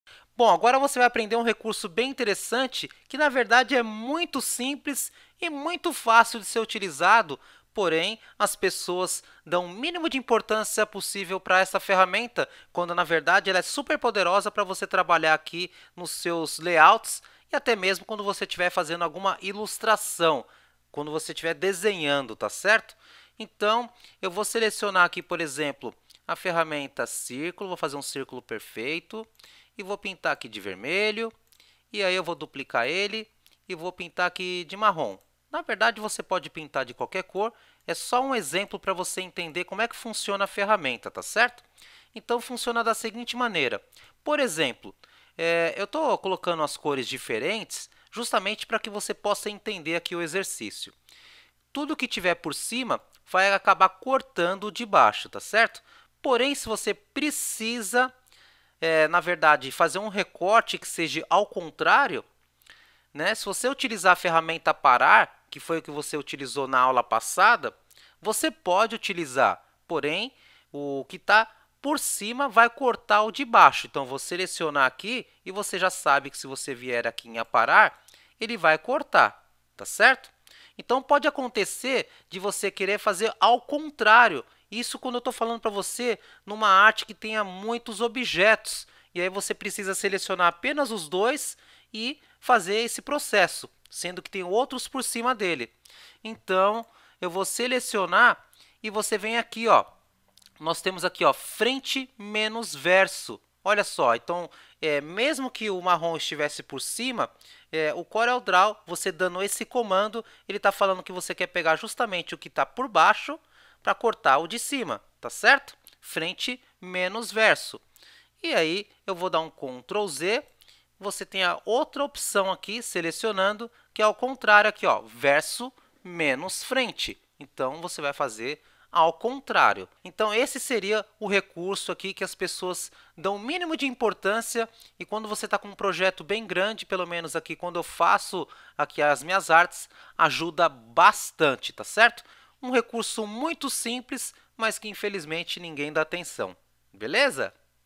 Curso de Sublimação para Gratuito - Aula 30 Ferramenta Frente Menos Verso
Hoje nessa aula do curso de corel draw para iniciantes você irá aprender sobre a ferramenta frente menos verso do Corel Draw.